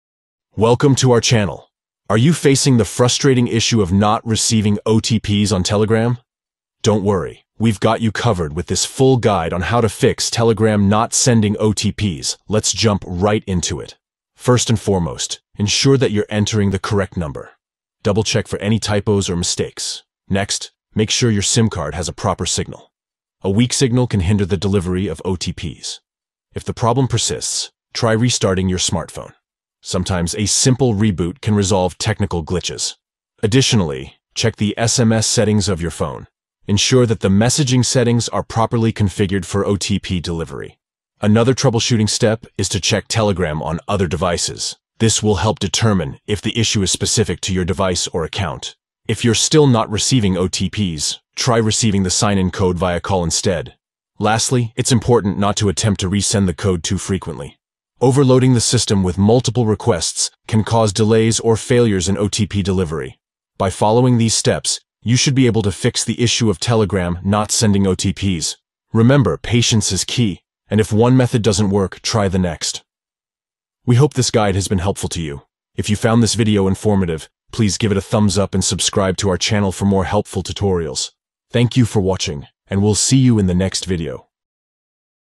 How To Fix Telegram Not Sending OTP - Full Guide
Having trouble with Telegram not sending OTPs? In this video, we provide a comprehensive guide on how to fix this issue. Follow these steps to resolve the problem:

1. Enter the correct number.
2. Ensure proper signal on your SIM card.
3. Restart your smartphone.
4. Check SMS settings on your phone.
5. Verify Telegram on other devices.
6. Receive sign-in code via call.
7. Avoid excessive code resending.

By following these steps, you'll be able to overcome the challenge of not receiving OTPs on Telegram. Stay patient and persistent, and you'll soon be back on track.